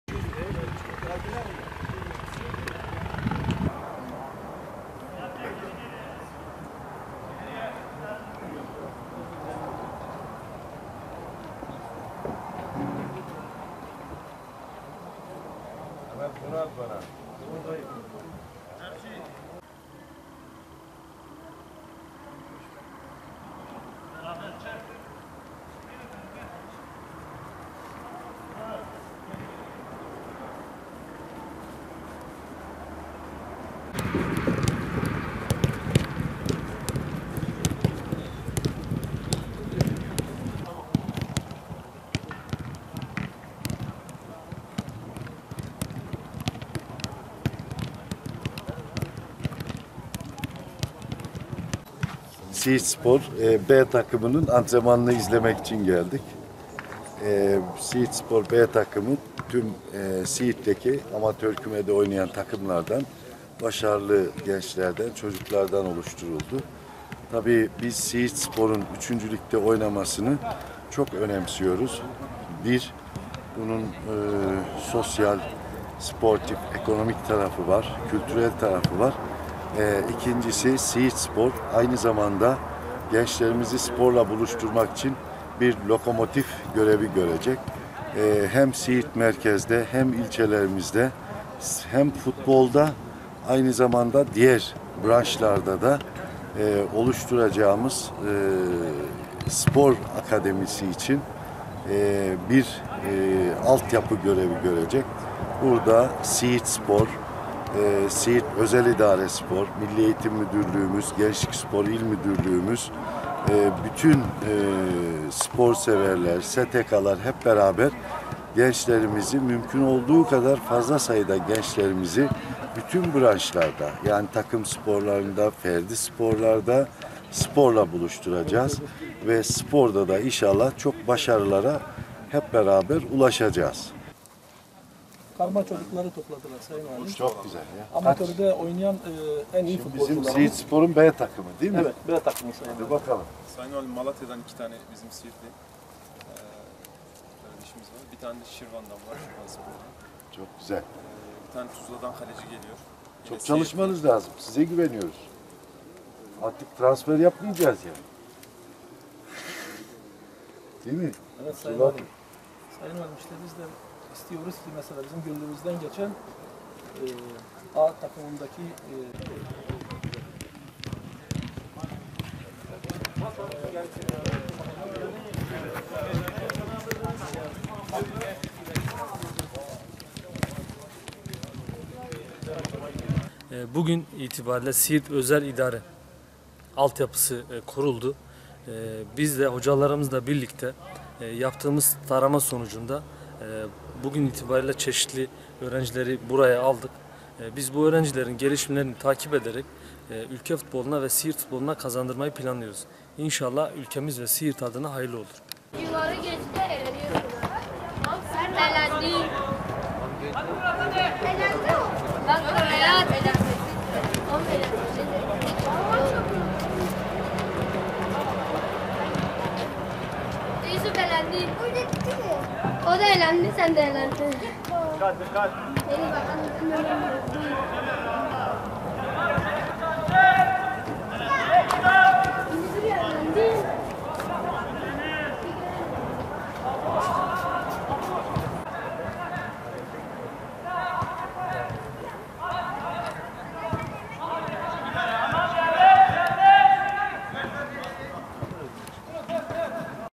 Siirtli gençler sporla yetişecek
Siirt Valisi ve Belediye Başkan Vekili Osman Hacıbektaşoğlu, Siirt İl Özel İdarespor B takımı futbolcularıyla bir araya geldi.
Siirt Valisi ve Belediye Başkan Vekili Osman Hacıbektaşoğlu Siirt İl Özel İdarespor tesislerini ziyaret ederek yeni kurulan altyapı çalışmaları hakkında Siirt İl Özel İdaresi Genel Sekreteri ve İl Özel İdaresi Spor Kulüp Başkanı Hıfzullah Canpolat’tan bilgi aldı. Siirt İl Özel İdarespor’un 3. Lig’de olmasından ötürü büyük memnuniyet duyduğunu belirten Vali Hacıbektaşoğlu, genç sporculara her türlü platformda destek vereceklerini söyledi. İl Özel İdarespor’un gençleri spor ile buluşturulacağını ve lokomotif görevi göreceğini dile getiren Vali Hacıbektaşoğlu, "Siirt İl Özel İdarespor B takımının antrenmanını izlemek için geldik. B takımı ilimizde amatör takımlarında oynayan başarılı ve tecrübeli genç çocuklardan oluşturuldu. Tabi ki Siirt İl Özel İdarespor’un 3. Lig’de oynamasını çok önemsiyoruz. İlk olarak bunun sosyal, sportif ekonomik, kültürel tarafı var, öte yandan Siirt İl Özel İdarespor aynı zamanda gençlerimizi spor ile buluşturmak için bir lokomotif olacaktır. Gerek Siirt merkezde, gerekse ilçelerimizde futbol ve diğer branşlarımızda oluşturacağımız spor akademisi için bir alt yapı görevi görecektir. Siirt İl Özel İdarespor, Milli Eğitim Müdürlüğümüz, Gençlik Spor İl Müdürlüğümüz, bütün sporseverler, STK’lar hep beraber gençlerimizi bütün branşlarda, yani takım sporlarında ve ferdi sporlarda spor ile buluşturacağız ve spor ile inşallah bu başarılara ulaşacağız. Siirt İl Özel İdarespor bu sene 3. Lig’de oynuyor bu başarıyı gösteren başta yönetime ve desteği bulunan herkese çok teşekkür ediyorum" dedi.

"Amacımız gençlerimizin kötü niyetli grupların tuzağına düşmelerini engellemek"
Siirt İl Özel İdarespor Kulüp Başkanı Hıfzullah Canpolat, A takımının kurulmasının ardından altyapı çalışmalarına başladıklarını  söyledi. Canpolat, "Amacımız ve hedefimiz ilimizde sağlam bir altyapı kurmak, çocuk ve gençlerimizin kötü niyetli birey ve gurupların tuzağına düşmelerini engellemek adına elimizden gelen her türlü çalışma ve özveriyi göstermekteyiz. Bugün itibariyle spor tesisimizde altyapı çalışmalarımız başlamış olup, gün geçtikçe geliştirerek çalışmalara hız verilecektir. Bu kapsamda altyapı B takımında 24 futbolcumuz olacaktır. 12-14 yaş grubunda ise 24 öğrenci, ilerleyen günlerde ise 10-12 yaş grubunda 24 öğrencimiz ile çalışmaya başlayacağız ve toplamda 72 öğrencimiz ile alt yapımızı kuracağız" diye konuştu.
Beden eğitimi öğretmeni Metin Karanfil ise "Bugün itibari ile Siirt İl Özel İdarespor alt yapısı kuruldu. Biz de hocalarımızla birlikte yaptığımız tarama sonucunda bugün itibariyle çeşitli öğrencileri buraya aldık. Biz bu öğrencilerin gelişimlerini takip ederek ülke ve Siirt futboluna kazandırmayı planlıyoruz. İnşallah ülkemiz ve Siirt adına hayırlı olur" şeklinde konuştu.